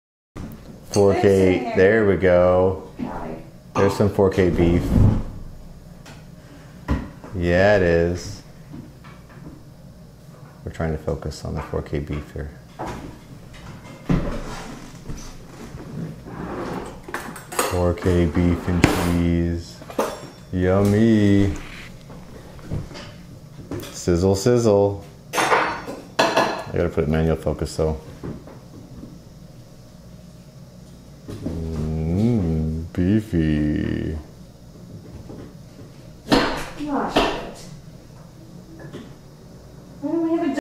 4K Beef! [Cheeseburger video]
trying out new 4k video equipment on grilled 4K beef!

Filmed with a Panasonic G85.